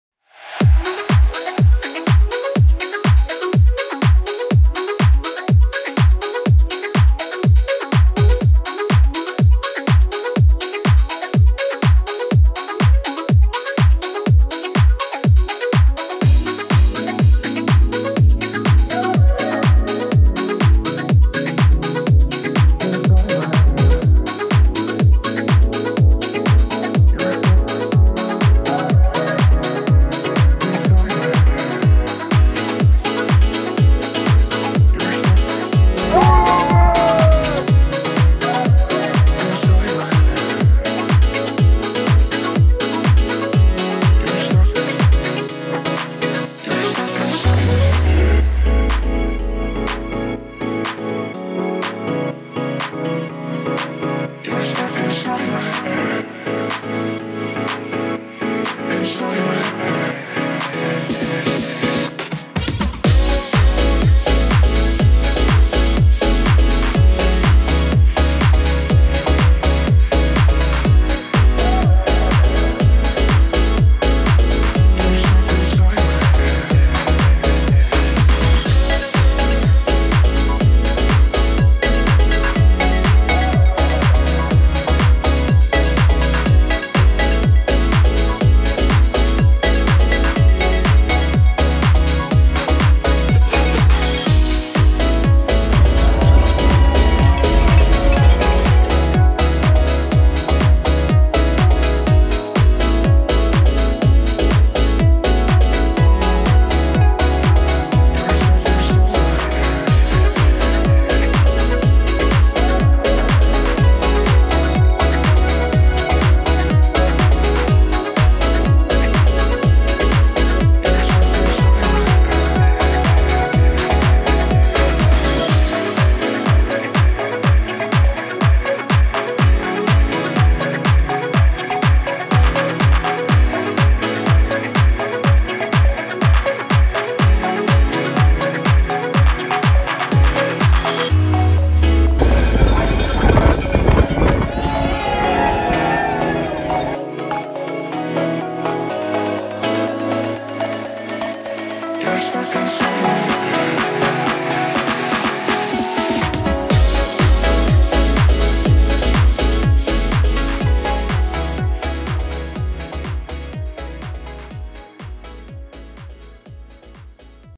Bolton Ironman UK 2021 Drone Video Highlights IMUK
Here's a little bonus video of Bolton Ironman 2021 with additional drone footage taken whilst filming for the official Ironman UK video   🏊‍♀️🚴🏃🎥🏆

Well done to all the athletes and a big thank you to the IronMan UK - Sheephouse Lane Fancy Dress Party wrestlers for bringing their A-game despite the rain! 👏🕺